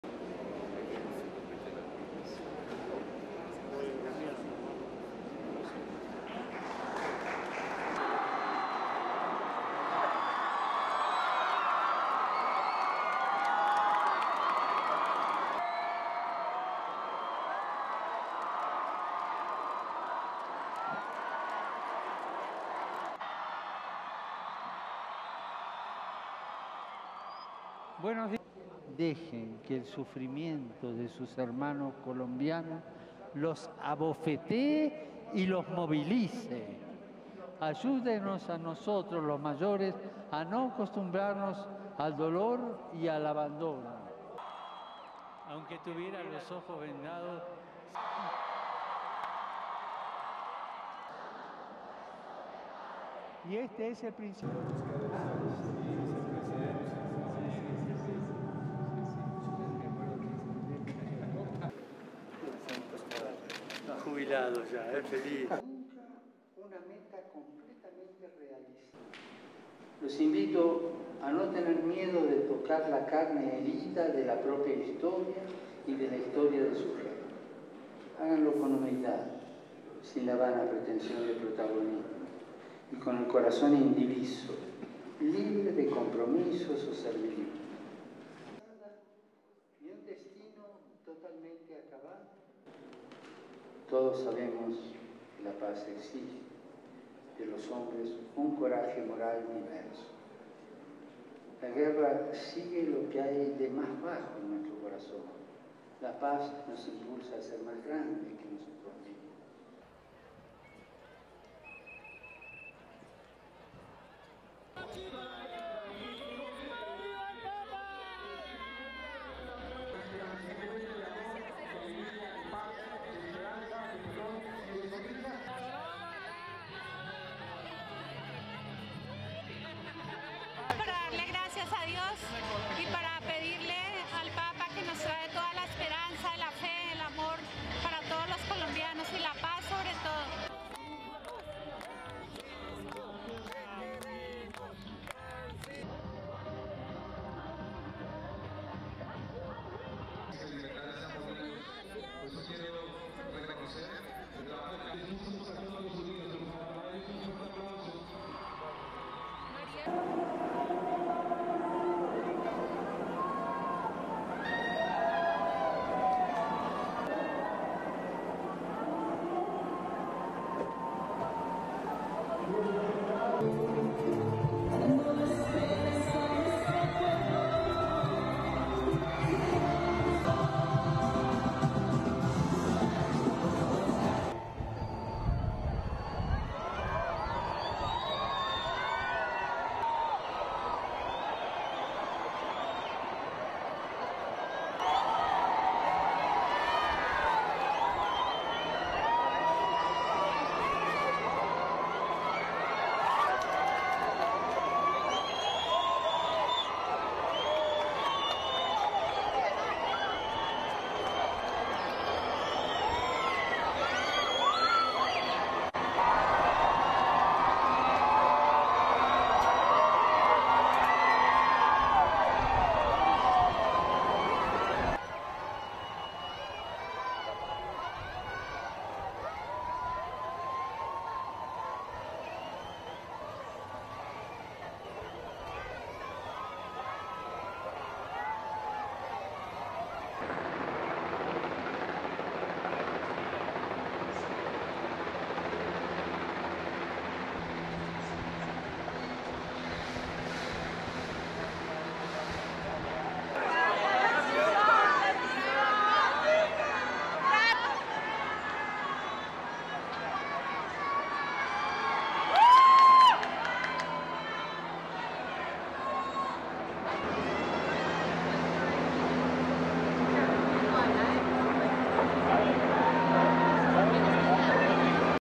Pope calls on bishops in Colombia to show courage for peace
(7 Sep 2017) Pope Francis is urging young Colombians to take the lead in promoting forgiveness to help their country heal from a half-century of armed rebellion.
Thousands of Colombians waving handkerchiefs in the red, yellow and blue of Colombia's flag greeted Francis in Plaza Bolivar, the main square facing Bogota's cathedral.
They interrupted him frequently with cheers, including when he repeated his famous line about how "only the young can make a mess!"
Francis praised young people for being able to move on from old resentments that "sicken the soul."
During his meeting with the bishops, Francis told them that they have a unique role to play in helping Colombians heal from a half-century of rebellion, saying they must show a "distinct kind of moral courage" to help Colombians overcome their base instincts of war and fear.
Francis also urged the church hierarchy to work for unity and communion in-house - a reference to the divisions even within the Catholic Church over the terms of the peace accord.
Many conservative opponents of the deal - including clergy - oppose the generous terms offered to the guerrillas.
In a speech Thursday at the residence of Bogota's archbishop, Francis urged Colombia's 130 bishops to give their flock the courage "in taking the first step towards definitive peace and reconciliation, towards abdicating the method of violence and overcoming the inequalities at the root of so much suffering."
Francis is stressing a message of reconciliation during his five-day visit to Colombia in hopes of solidifying the country's year-old peace accord that is still bitterly contested by many Colombians who oppose the generous terms offered the rebels to lay down their arms.